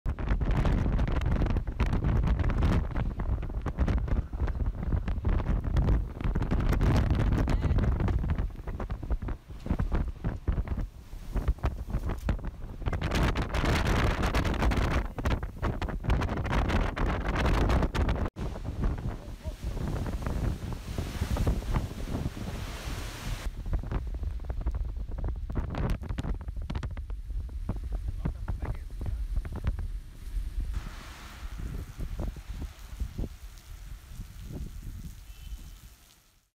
Monster cyclone smashes eastern India
Cyclone Fani, one of the biggest storms to come off the Indian Ocean in recent years, slammed into India on Friday, tearing down trees, blowing away food stands and cutting off power and water. The monster weather system, which threatened to bring a dangerous storm surge that could flood low-lying areas, was packing winds of up to 180 kilometres (110 miles) an hour as it ripped into the country's east coast.